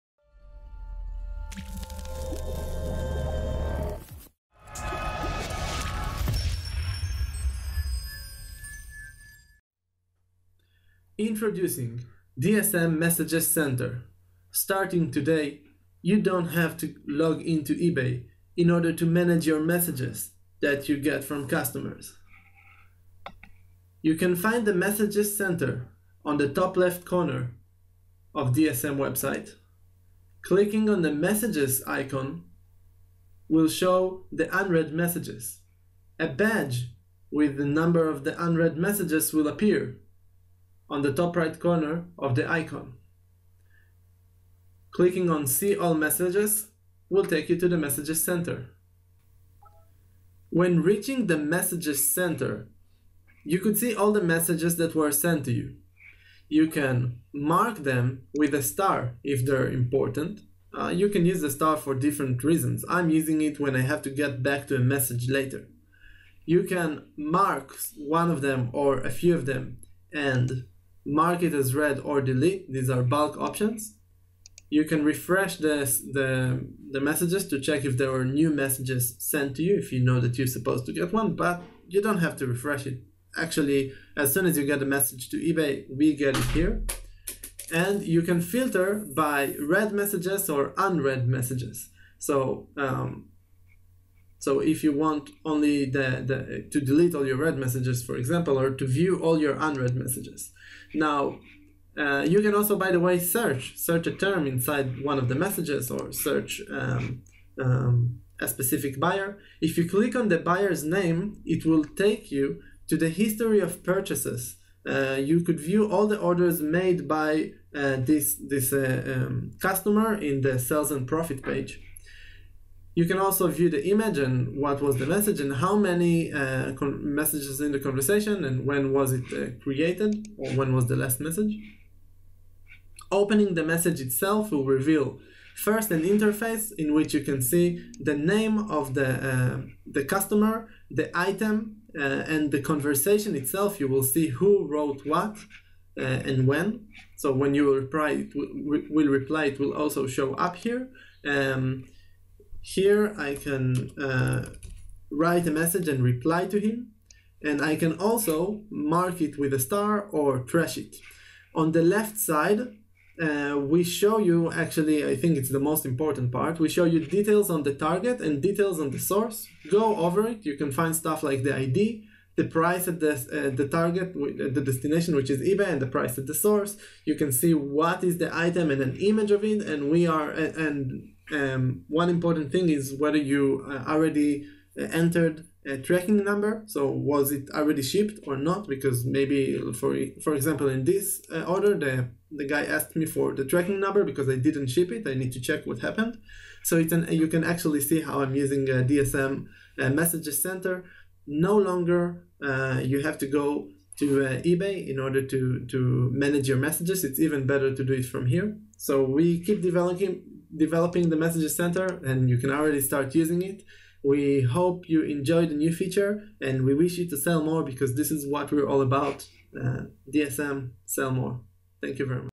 DSM Tool Messages Center
Introducing the DSM messages center. Now you can manage your messages in eBay without exactly logging in to eBay. You can manage them straight from the DSM tool platform. Watch the video to show you how it is done.

Learn what you need to know about drop shipping at the Dropship Academy: